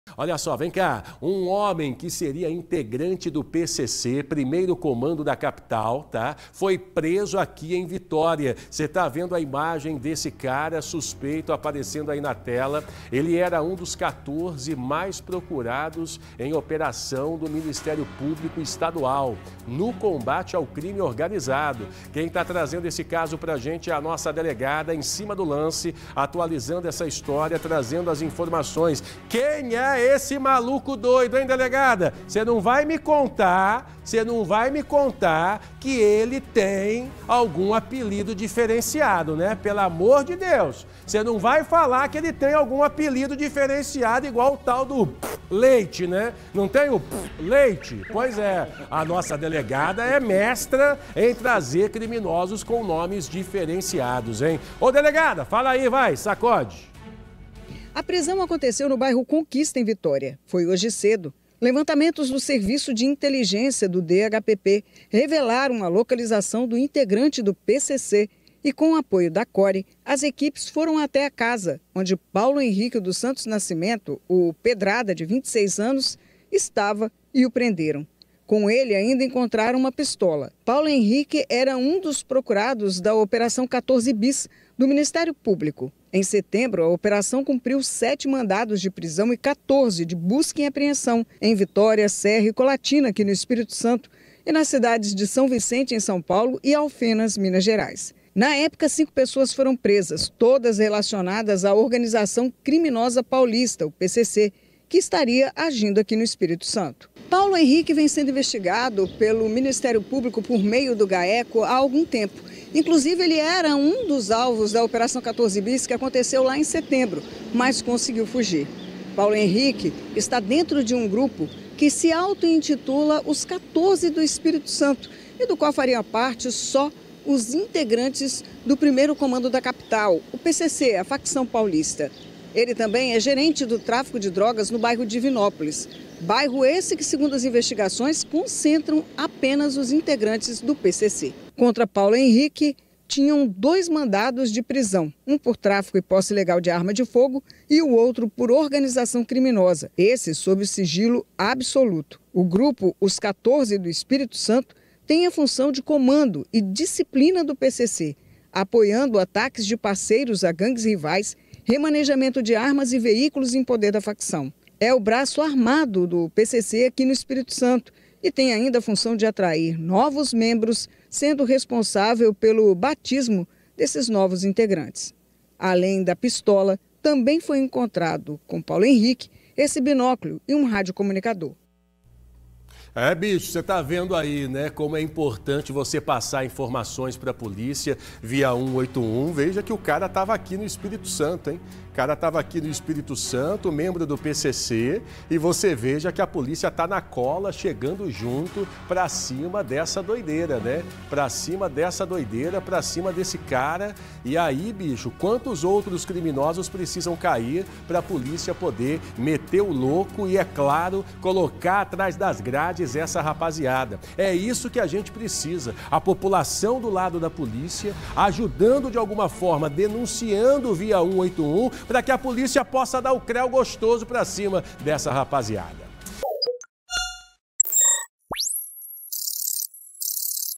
Integrante de facção criminosa é preso em Vitória
Suspeito é acusado de ser gerente do tráfico de Divinópolis, em Minas Gerais.